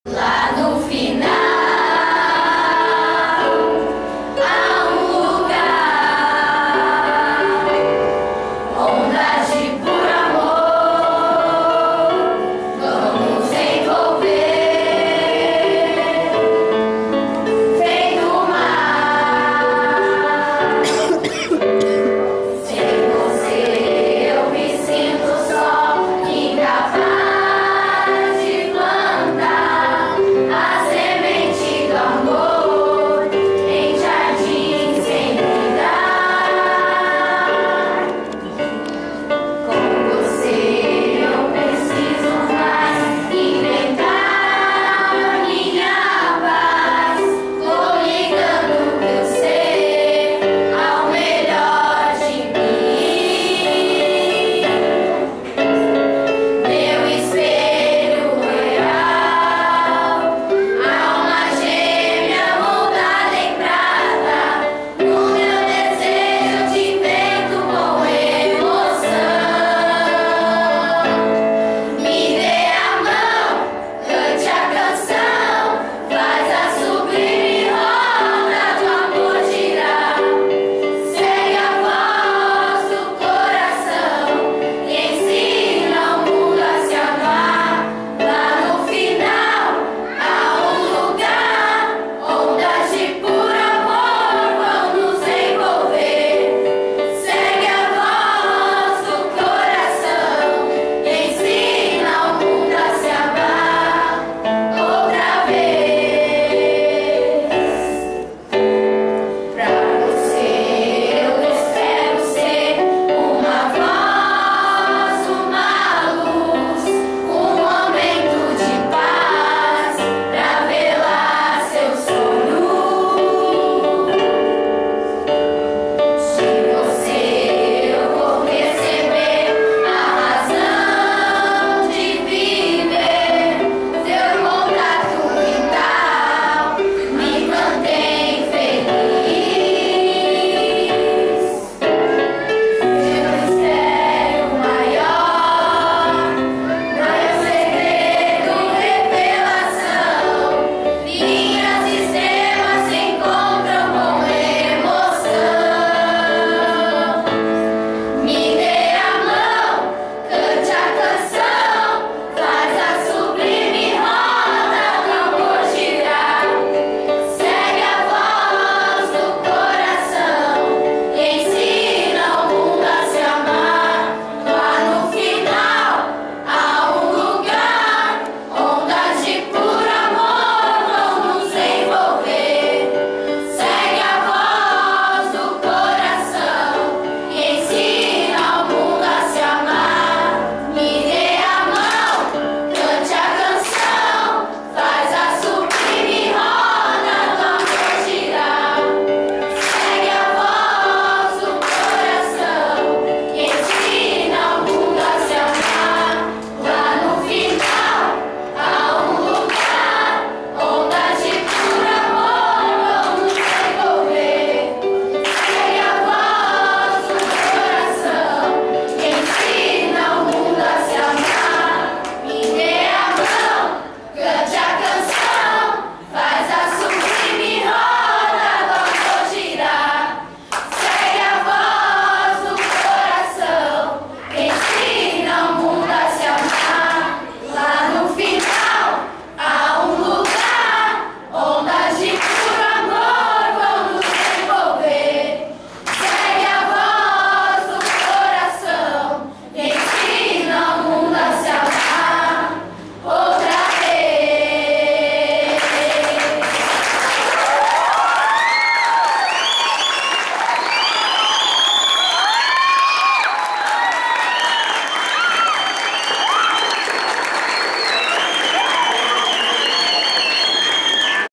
Escola em "concerto" E.E. José Carlos da Silva Júnior - Caieiras
Projeto Orquestra, parceria entre a Prefeitura de Caieiras e a E.E. José Carlos.